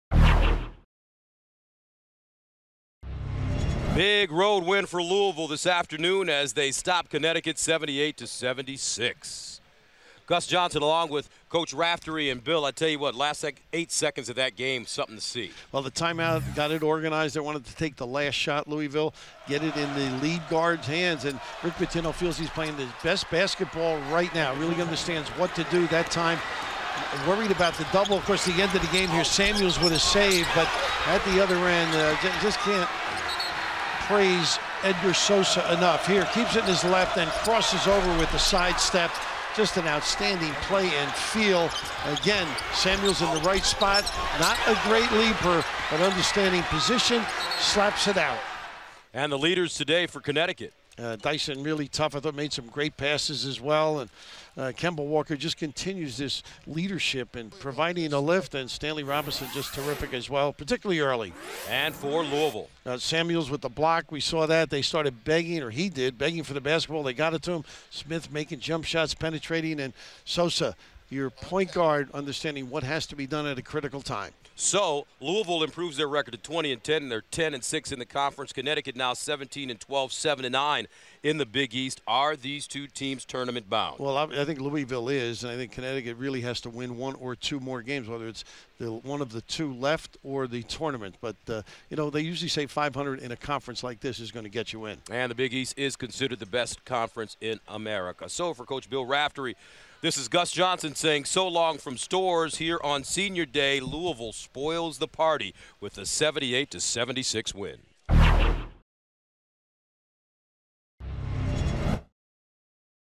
Louisville vs. Connecticut Recap
Edgar Sosa's driving layup with 8 seconds left gave Louisville a 78-76 victory over Connecticut on Sunday. Gus Johnson and Bill Raftery recap the stunning finish in Storrs, Connecticut.